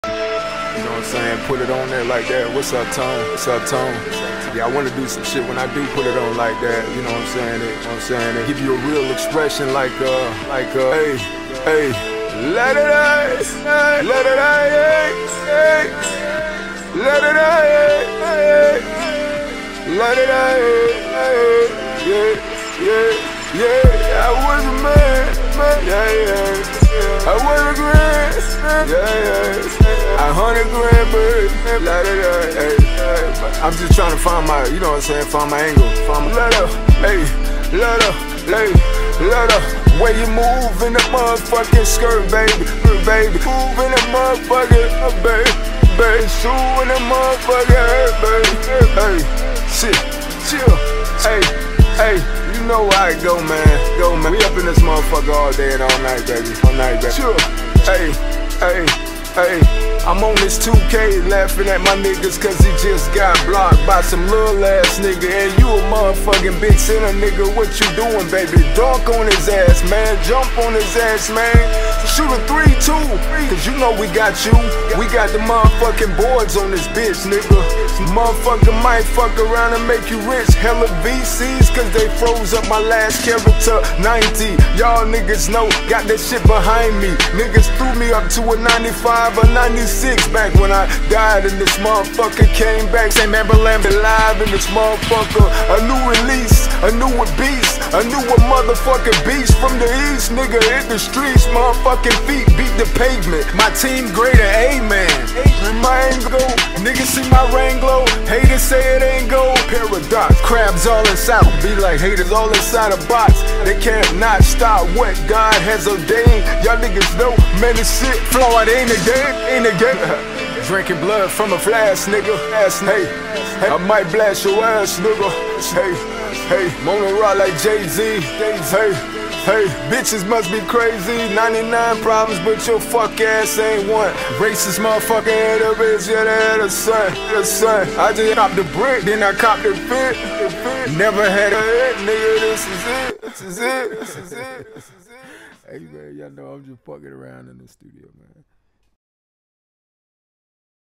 NBA 2k Freestyle
Just a quick freestyle I did when 2k accidentally deleted my character and restored him at a 69 overall with VC which still wasn’t enough to get back to where I was and what I lost. But that life we take loses in life and we eventually bounce back. Stay grinding stay working hard!